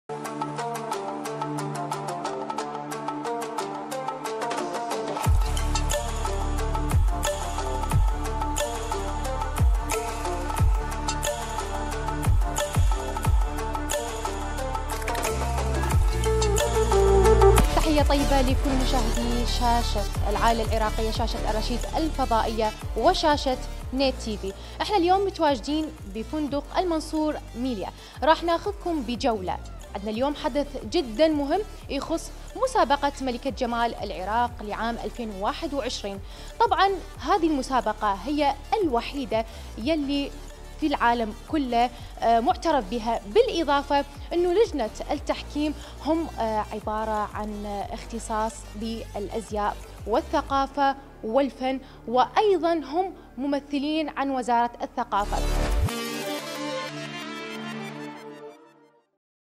Exclusive scenes from Miss Iraq 2021 | Maria Trading
Exclusive scenes from the stages of choosing Miss Iraq for the year 2021
كواليس حصرية من مراحل اختيار ملكات جمال العراق للعام 2021